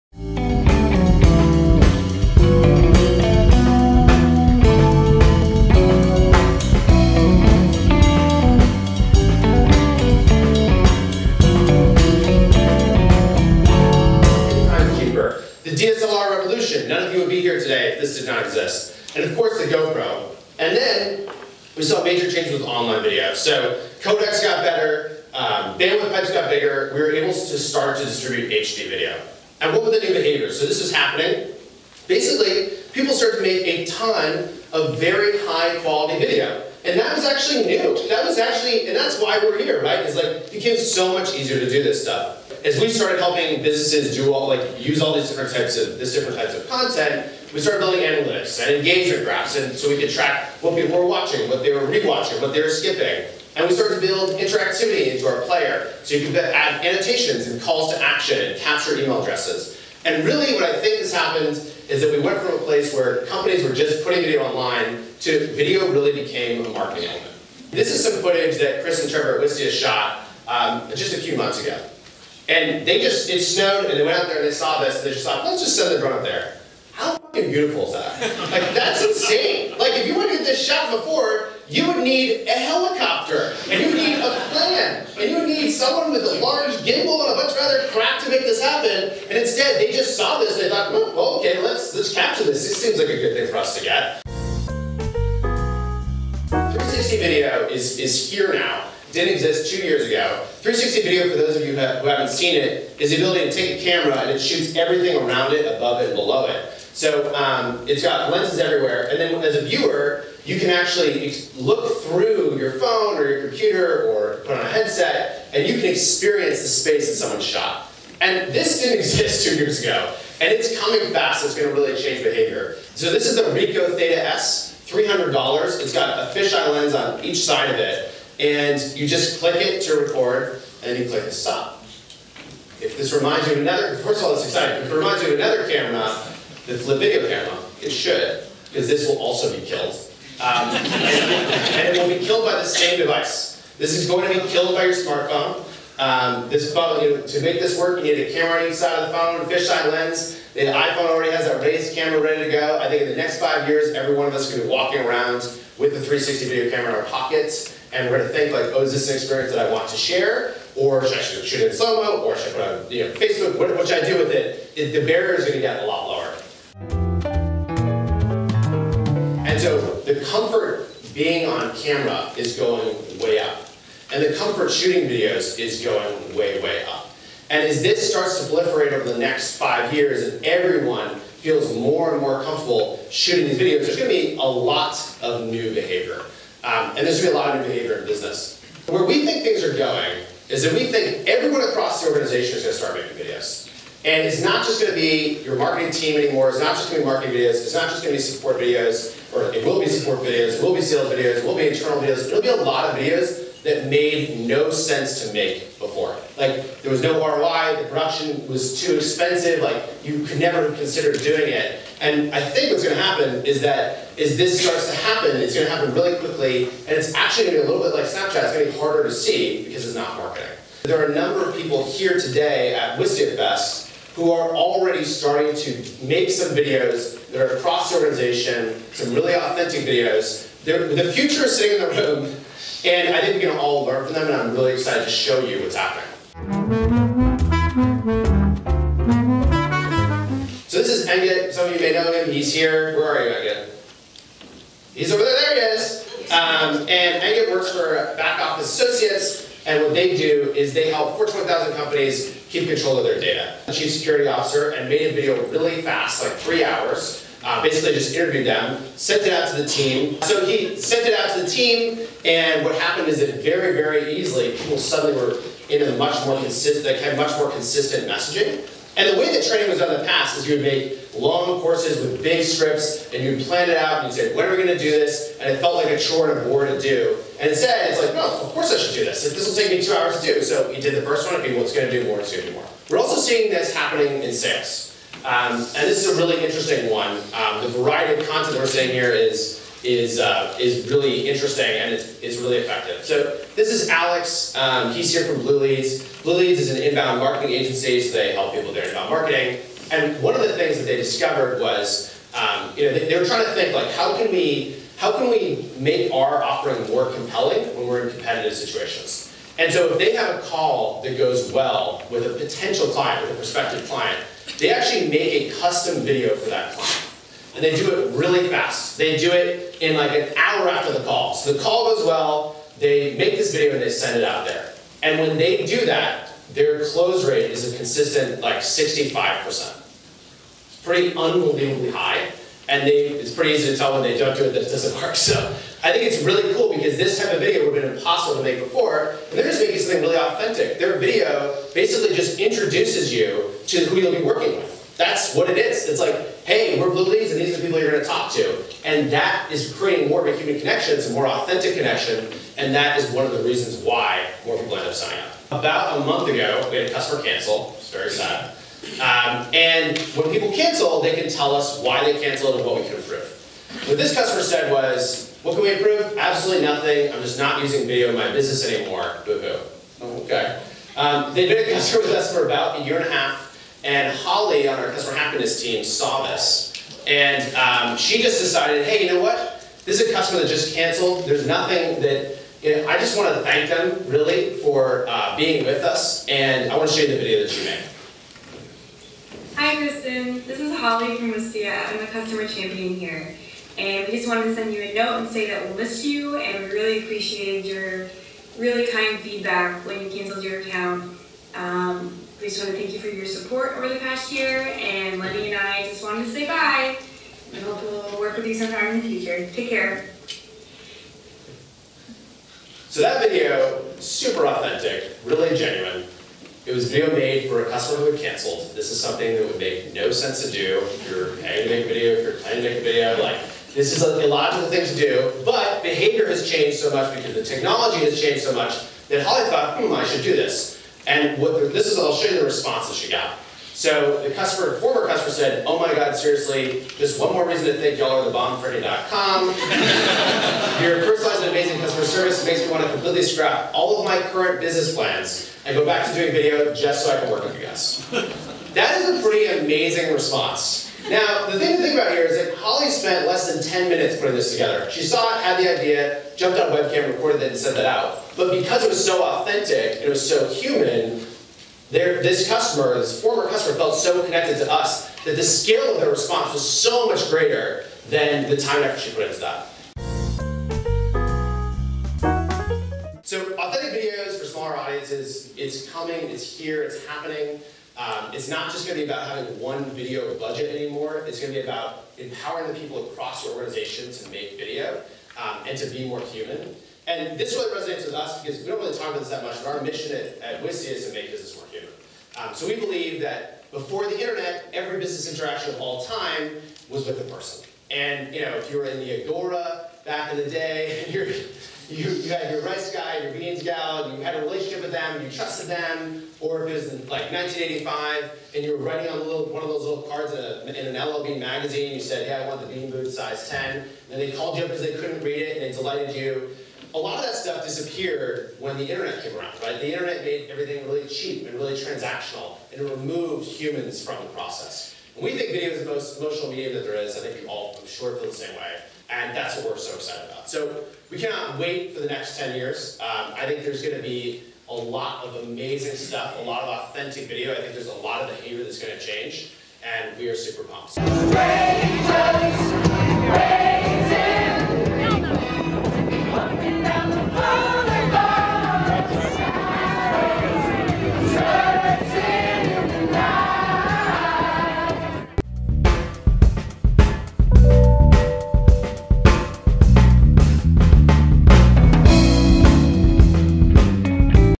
Best of the Fest: Wistia CEO Chris Savage's Keynote
Did you miss WistiaFest 2016? We have the best segments of Wistia CEO Chris Savage's keynote presentation here. Check it out! Video marketing is evolving.